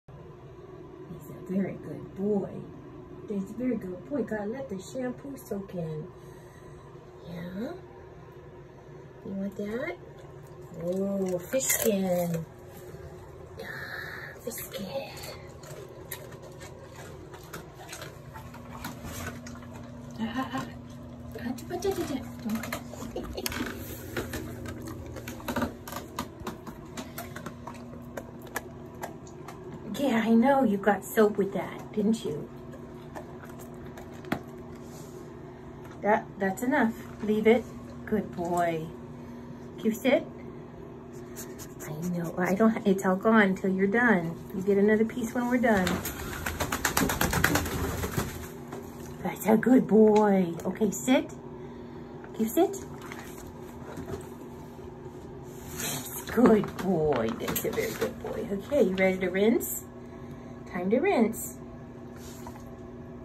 Bath Time for Rufus
Rufus loves splashing around in water but bath time doesn’t count.